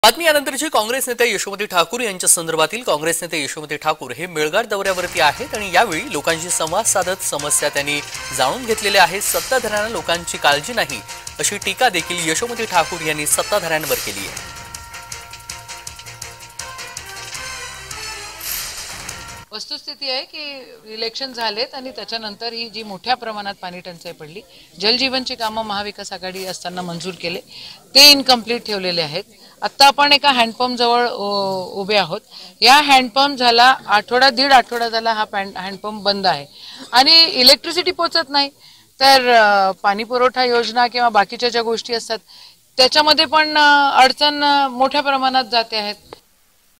Yashomati Thakur | काँग्रेस नेत्या यशोमती ठाकूर मेळघाट दौऱ्यावर | Marathi News
काँग्रेस नेत्या यशोमती ठाकूर मेळघाट दौऱ्यावर
'लोकांशी संवाद साधत समस्या ऐकल्या'